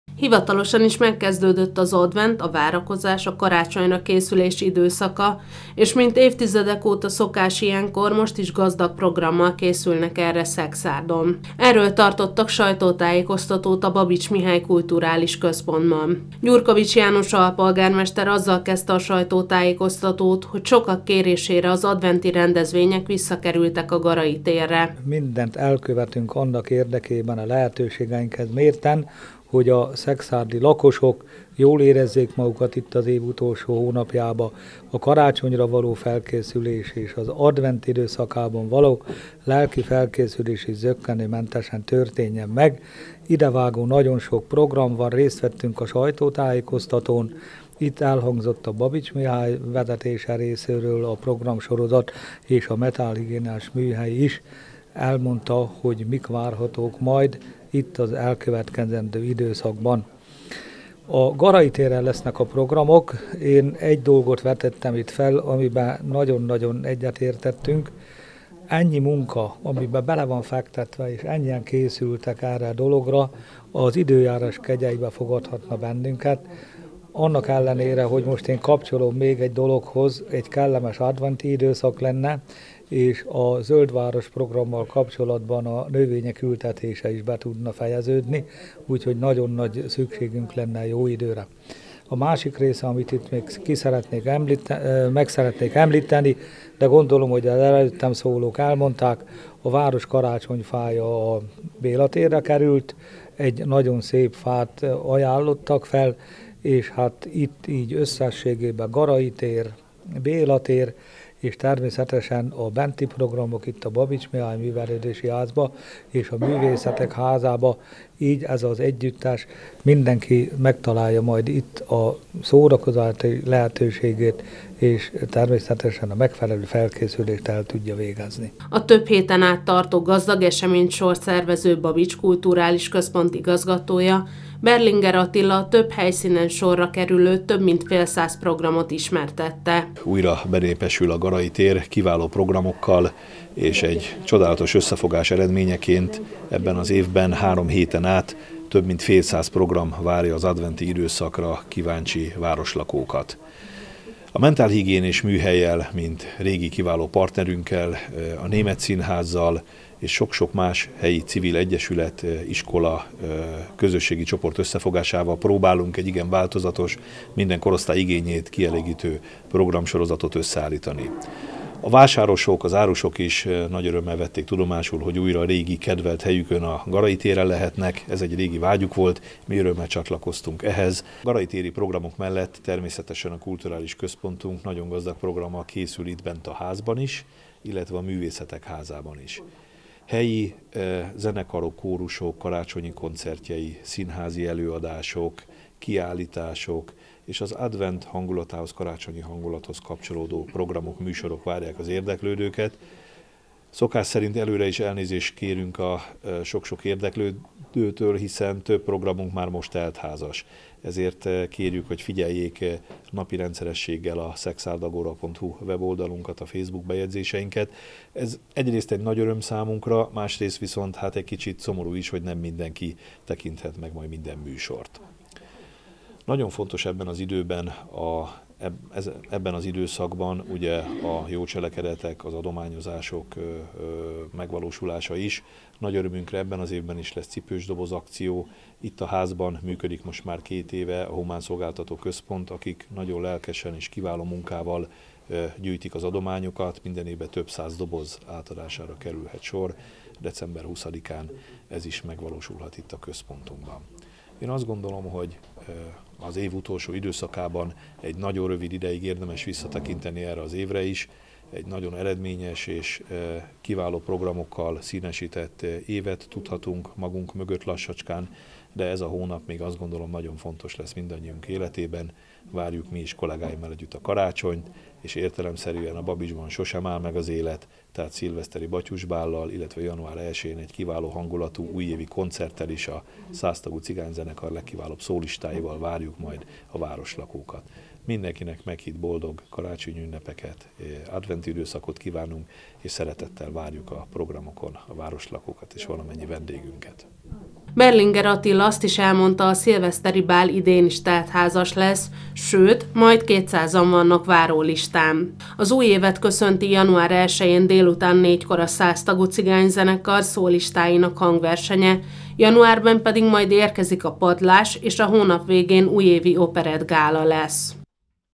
Rengeteg programot kínál a vármegyeszékhely
Hivatalosan december 1-én pénteken vette kezdetét a Szekszárdi Advent 2023, majd első gyertya meggyújtásával folytatódott a több hetes programsorozat a vármegyeszékhelyen. A részleteket a Babits Mihály Kulturális Központban tartott sajtótájékoztatón ismertették.